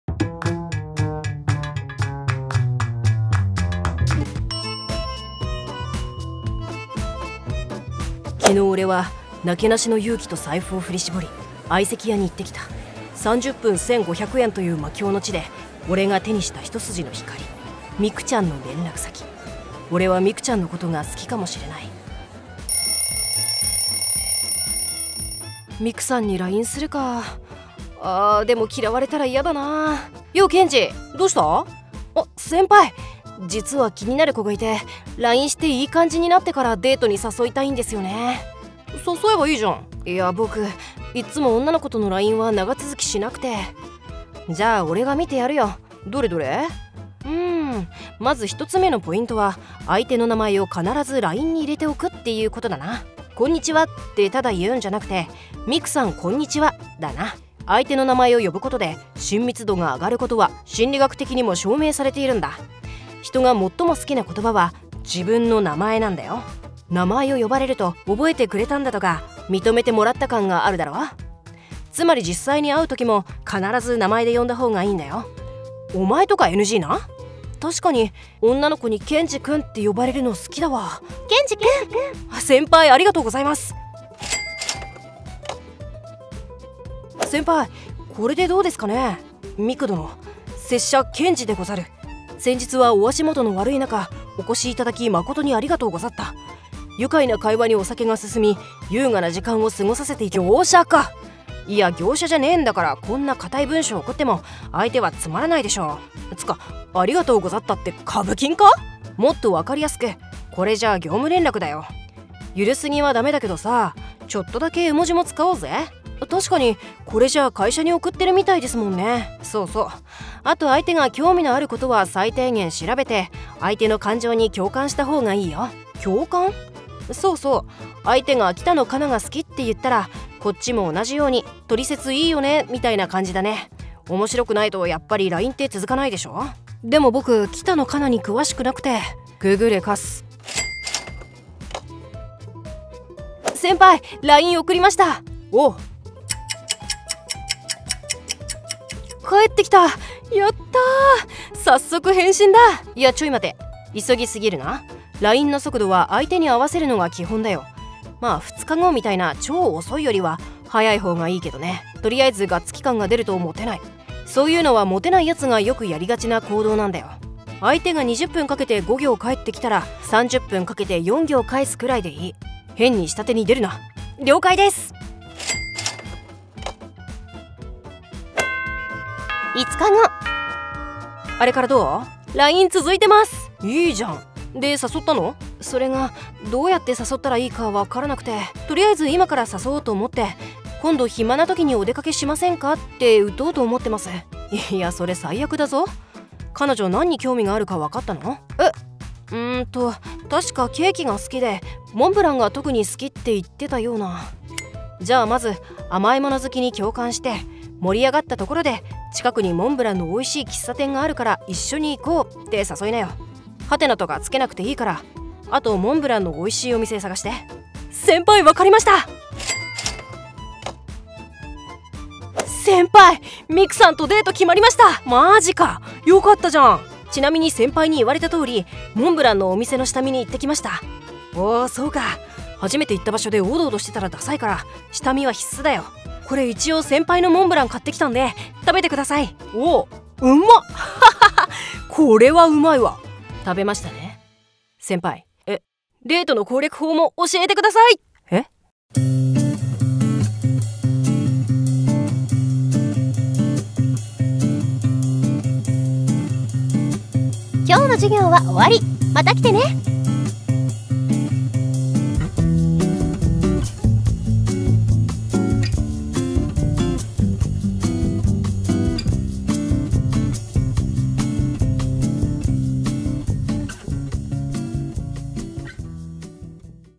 【漫画】モテる男性のLINEテクニック５選【マンガ動画】
『俺は昨日、勇気と財布を振り絞り、相席屋に行って来た。』
俺はミクちゃんのことが好きかもしれない・・。

当チャンネルに掲載されているコンテンツ（文書、映像、音声等）は、オリジナルの動画であり、著作権法、関連条約・法律で保護されています。

〜音楽〜
甘茶の音楽工房